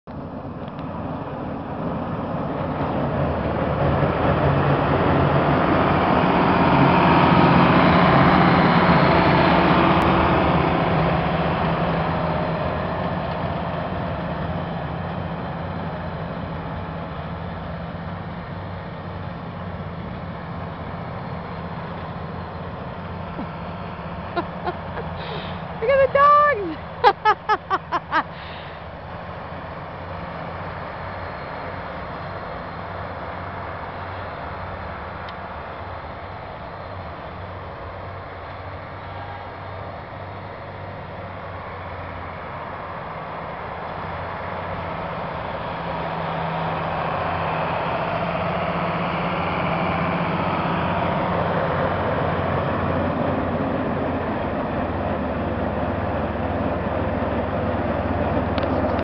Now THIS is a snowblower!
1586 International, with a duel fan, nine foot wide front-mounted snowblower;  with stacks that are 10 feet, from ground to shoot.   Snow can be blown up to 150 feet!  It's so powerful and makes quick work of a farm yard.  This 1586 was purchased new in 1978, and is used frequently; summer and winter.    In Minnesota, snow is a weekly occurrence, and this beauty is a keeper.  Perhaps this got a little too close to the Metrodome? Ha ha!   (Mall of America Field,)